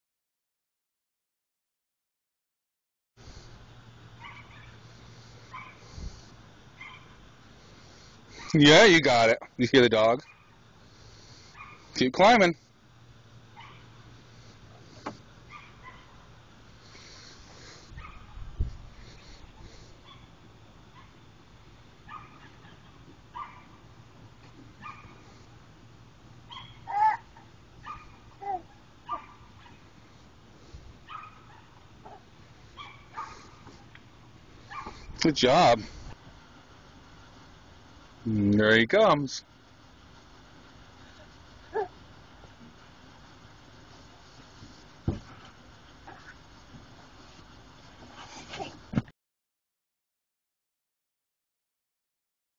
Ladder climbing surprise
2 1/2 year old Treyton has mastered the ladder on his backyard slide and shows off his stuff. This is a big deal for a kid with Down syndrome and way ahead of the schedule the therapist set for him. You can tell he was under his dad's supervision by the way things ended.